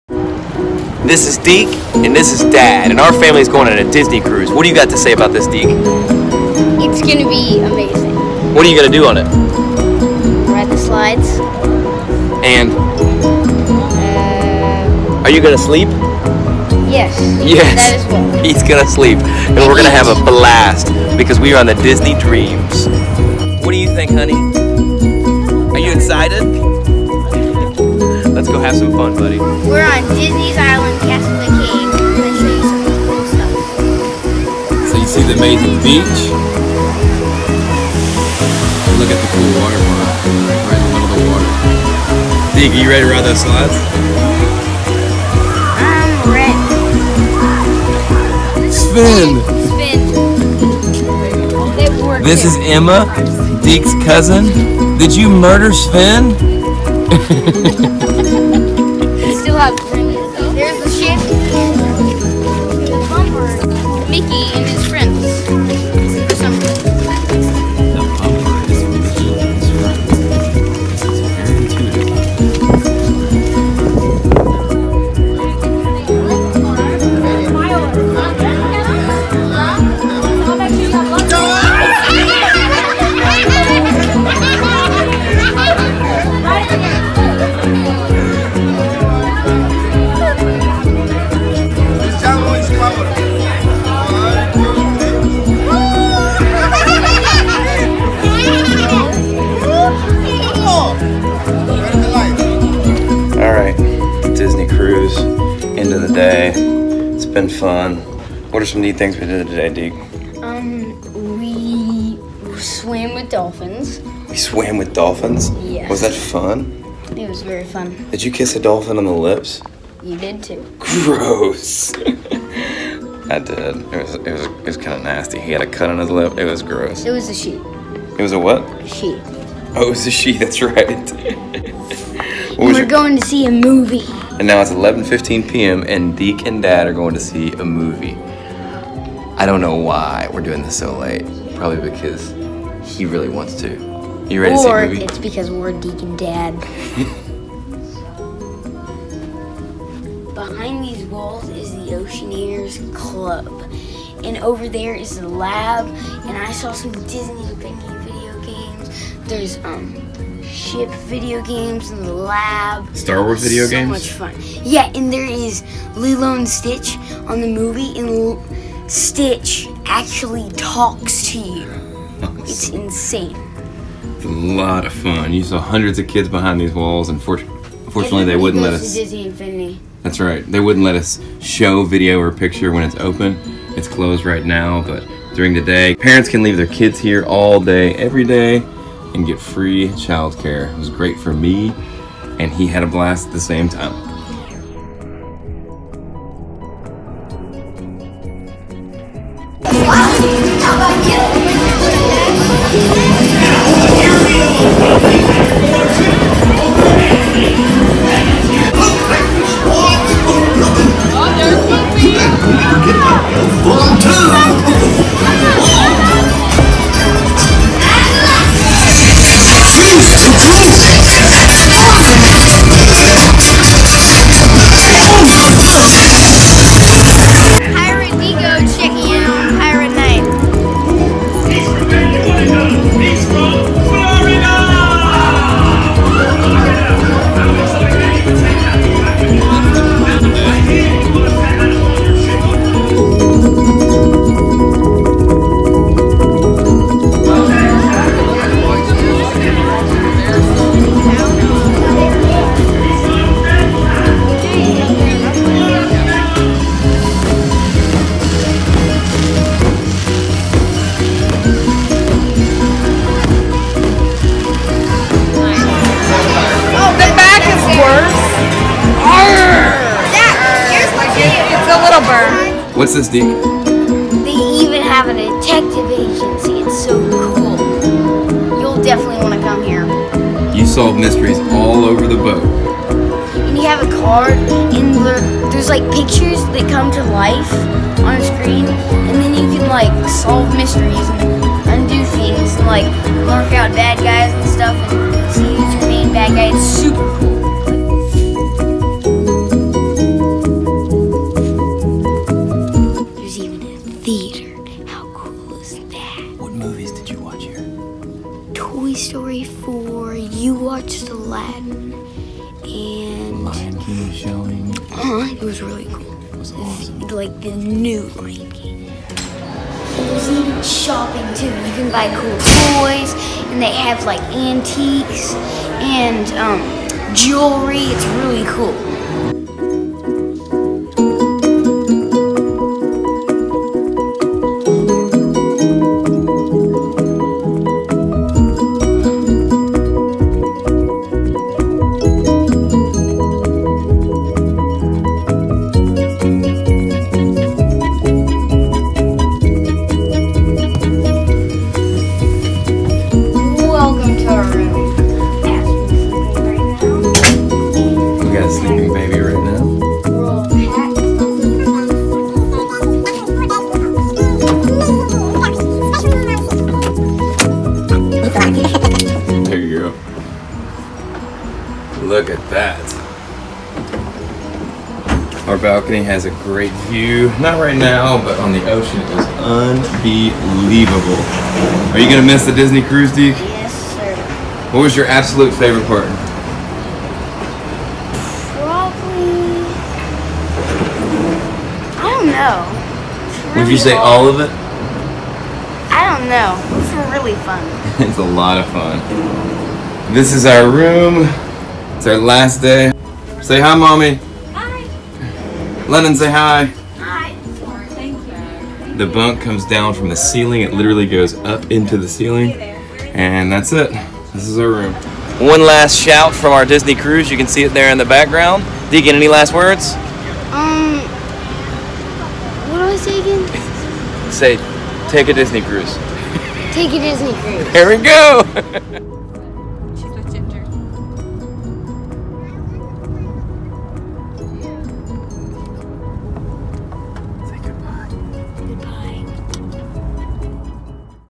DEAK ‘N DAD go on a Disney Cruise
We went on the Disney dream. It was a dream! Watch here for highlights of the trip and the ship.